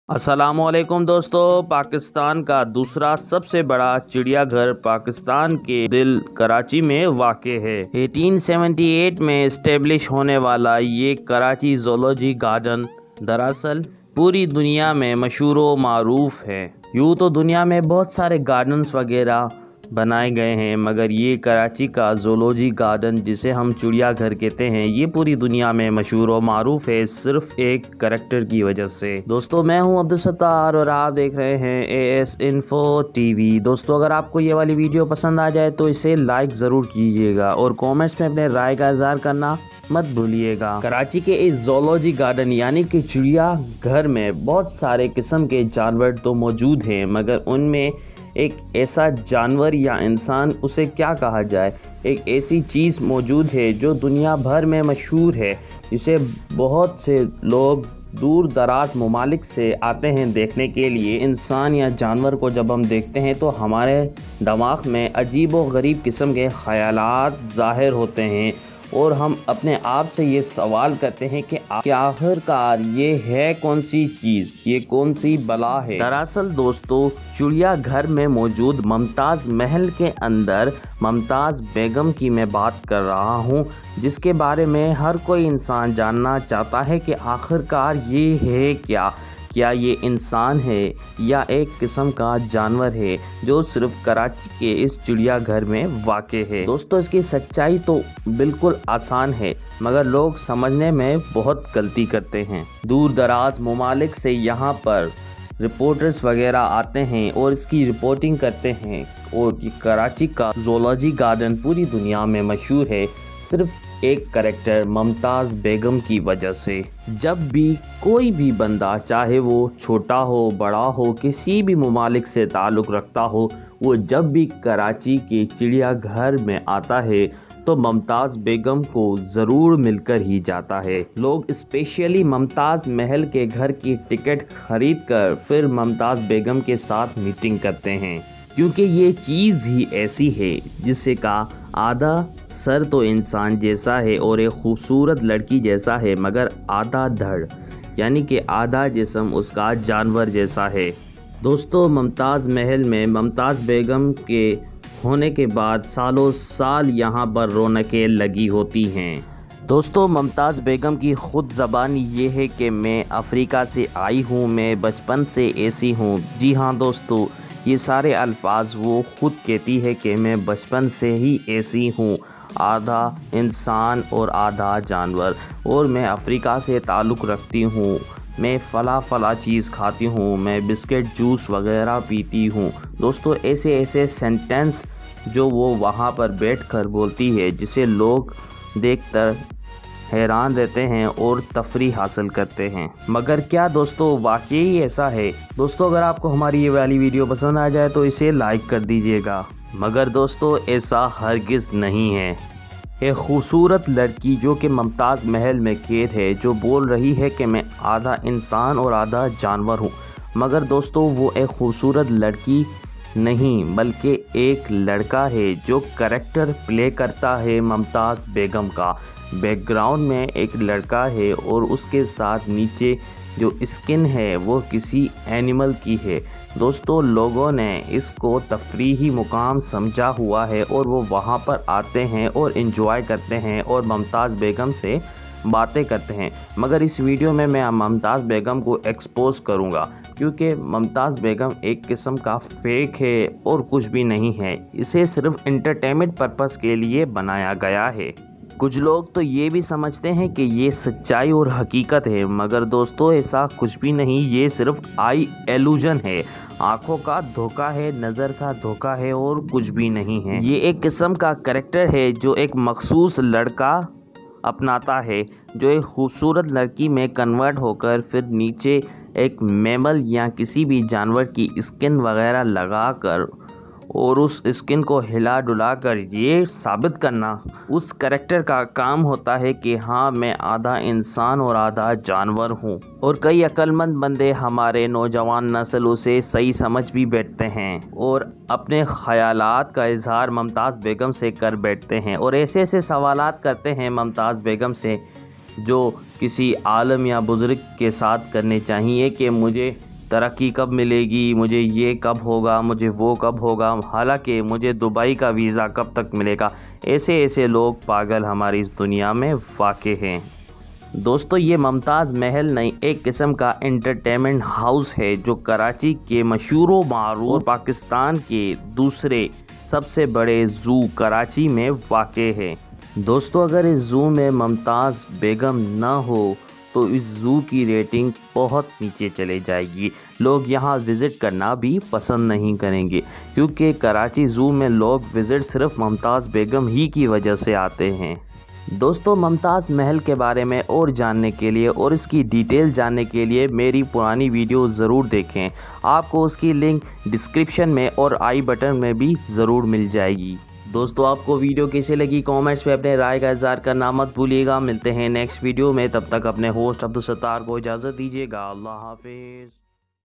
Reality Of Mumtaz Begum | Mumtaz Begum In Karachi Zoo| Mumtaz begum ki haqeeqat | AS INFO TV
Reality Of Mumtaz Begum | Fox Woman In Karachi Zoo | Mumtaz begum ki haqeeqat real story of fox-lady

The Video is about fox woman known as mumtaz begum in Pakistan 2nd largest  Karachi zoo known as Chidiya Ghar Karachi,
I exposed the reality of mumtaz begum that it is totally eye illusion.
Watch. What is reality of mumtaz begum?
today we will introduce Mumtaz Mahal located inside the Karachi Zoo. Karachi’s zoological gardens (also called Gandhi Gardens) has many animals to boast about
For the past 35 years a ‘maa-fauq-ul-fitrat’ (super natural) character is also kept in captivity here. This creature has a head of a lady and body of a fox. It is kept in a special pavilion called Mumtaz Mahal.

Inside the zoo, it is easy to find Mumtaz Mahal as the organisers use megaphone to invite you to see it. Zoo’s entrance ticket does not cover the visit to Mumtaz Mehal therefore one has to buy a separate ticket to see it.

Once inside the pavilion, you will see a fox’s hide placed on a table. Suddenly a woman’s head appears out of this hide in bright lipstick and heavy make-up. Small children in the audience are usually spell bound at this stage. The creature then talks with following starting line:

I was born like this in Africa 35 years ago, and live on cake, juice, and fruits …

This woman-fox creature also tells visitors their destiny and can allegedly answer your questions in any language of the world. I’ll leave the proof of this claim to our readership, who should visit Mumtaz mahal and ask this creature a question themselves. I’ve read on a website that people ask this creature all kinds of questions about their destiny. Somebody even asked about when will he get a work visa for Dubai.

The reality of Mumtaz Mehal is of course a big joke or hoax. The lady face is actually an enunch who has been doing this job for many years. The effect of fox body is created by optical illusion using mirrors. One thing I do want to mention is eventhough I wrote off Mumtaz Mehal as a big joke, it is still very popular among visitors.

I also have many fond memories of going to the zoo for day field trips with my primary school classes and family. I must admit, at younger age I used to be scared to go to Mumtaz mahal and look at this creature.

Following distant shot of Mumtaz Mahal pavilion shows the complete name of this creature as ‘Mumtaz Begum Africa wali.’

My Old Video About Mumtaz Begum.

Music Credit.

Like At Fb..

Allah Hafiz .